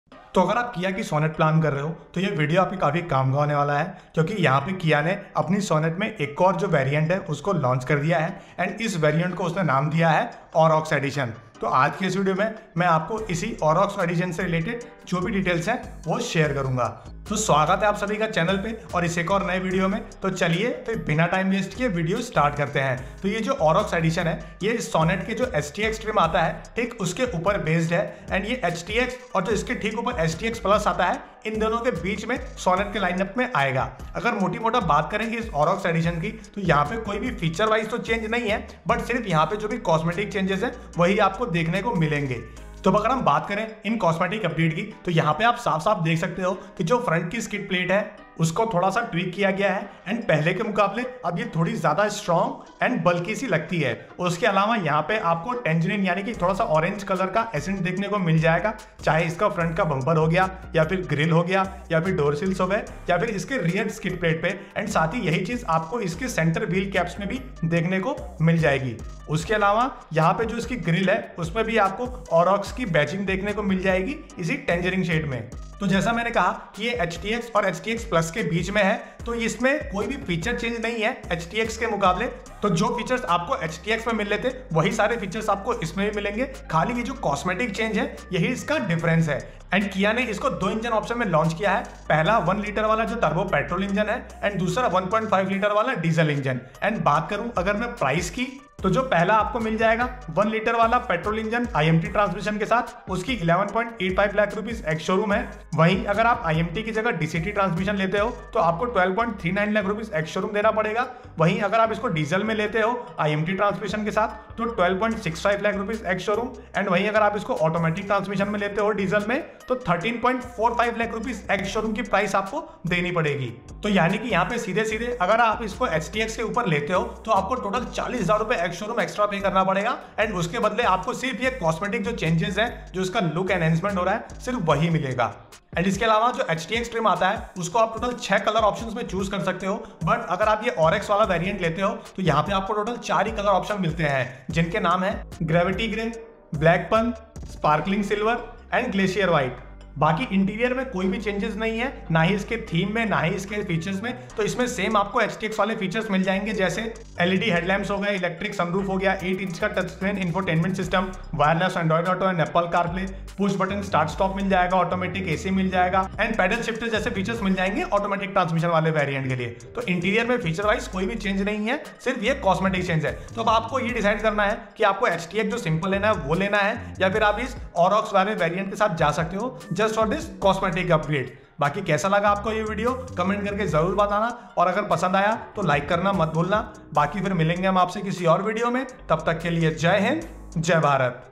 Kia Sonet 2023 Aurochs Edition: What's New and Worth the Hype | Better than HTX?
Kia Sonet Gets A New ‘Aurochs’ Edition; Priced From Rs 11.85 Lakh. The changes to the Aurochs Edition are just cosmetic. Up front, you get a tweaked skid plate design with tangerine accents. The same finish is seen on the grille and there’s the exclusive ‘Aurochs’ badging as well. It gets the same 16-inch alloy wheels but with the tangerine wheel cap surround. The side profile is further enhanced by a new skid plate with tangerine door garnish. The rear skid plate has also been tweaked with the redesigned which gets the tangerine accents.
While the HTX variant can be had in six shades, this edition is available only in four options - Gravity Grey, Aurora Black Pearl, Sparkling Silver, and Glacier White Pearl.
No changes have been made to its interior theme, as it continues with the same black and beige interior with leatherette seats.
No features have been added additionally to the Sonet Aurochs edition. It continues with LED headlamps, an electric sunroof, an 8-inch touchscreen system with wireless Android Auto and Apple CarPlay, push button start-stop, automatic AC, and paddle shifters (only for automatic variants).

Safety is covered by four airbags, electronic stability control, rear parking camera, hill hold assist, and traction control.